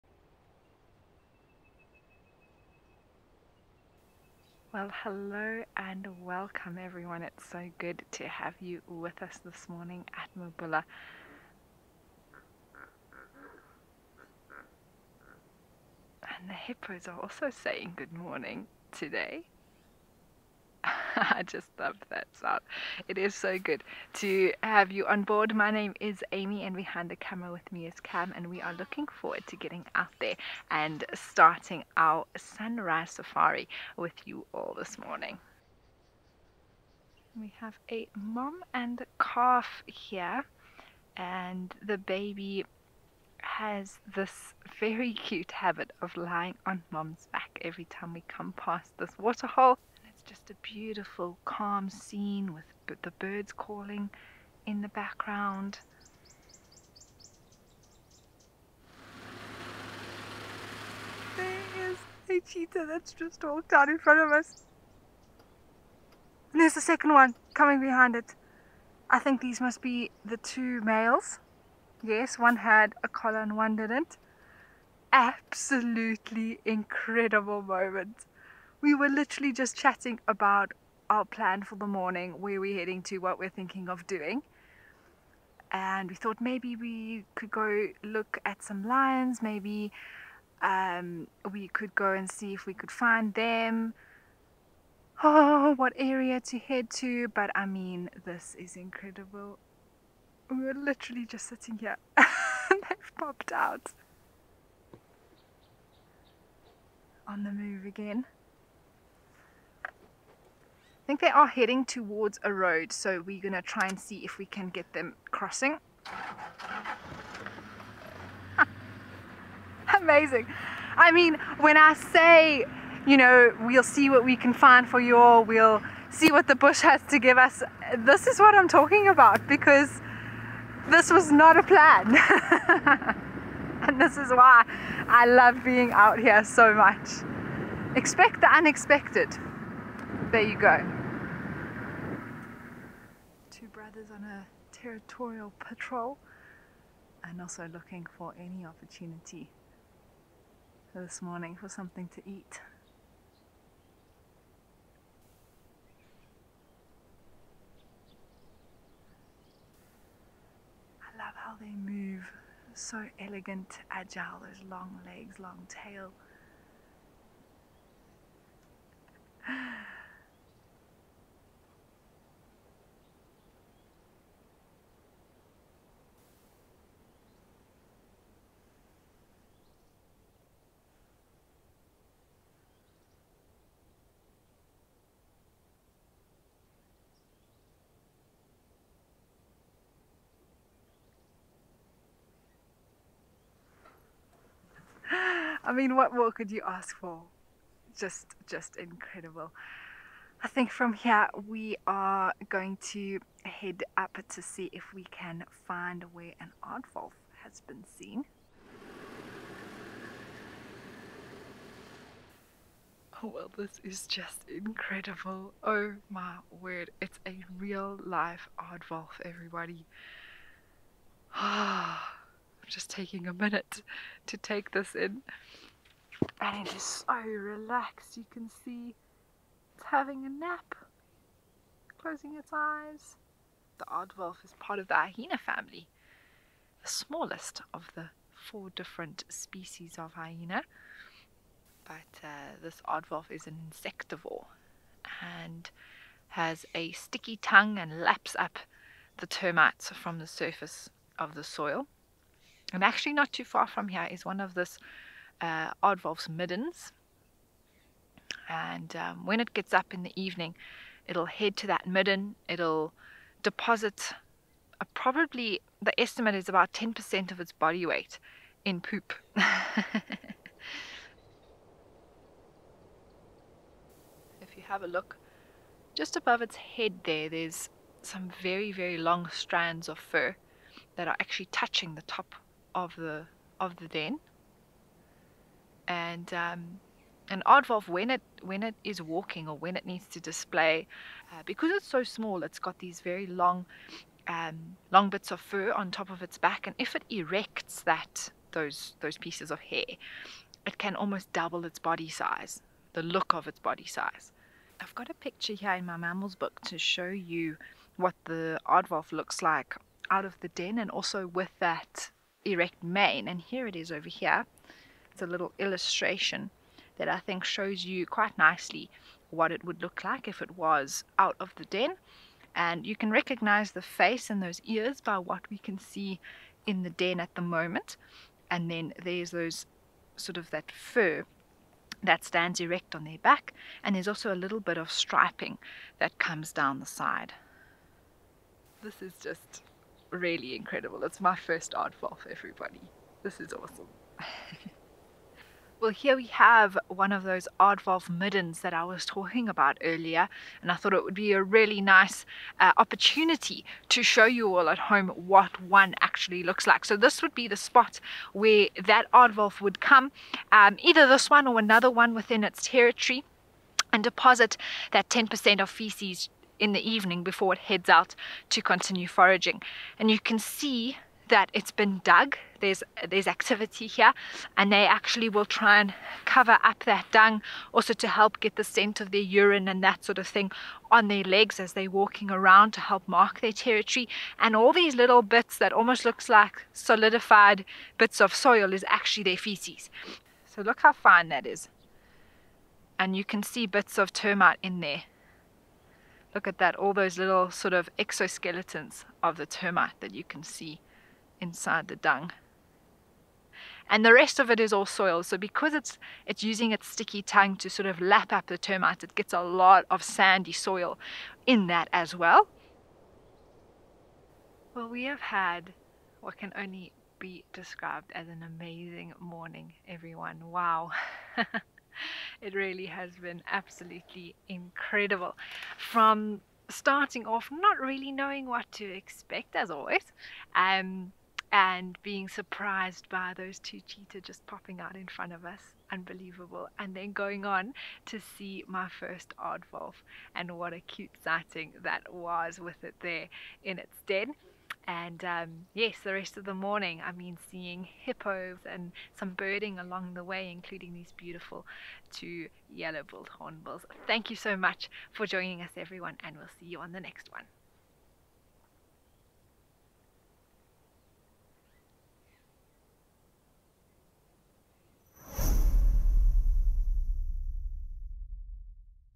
Cheetahs and aardwolf hunt the plains of Mabula
Step into the heart of Mabula, where nature's wild wonders await. Lounging hippos in the sunrise and then the fragile grace and beauty of Mabula's resident cheetah brothers. These are two remarkable males, one of which has a striking story of resilience. Despite missing an eye, this resilient predator remains a successful and adept hunter, embodying the spirit of survival.

The excitement doesn't end there! After the cheetah, the team found a very relaxed aardwolf, demonstrating the incredible diversity of Mabula's wildlife.

This nocturnal creature, usually shy and elusive, offers a unique glimpse into its world, adding another layer of fascination to your safari adventure.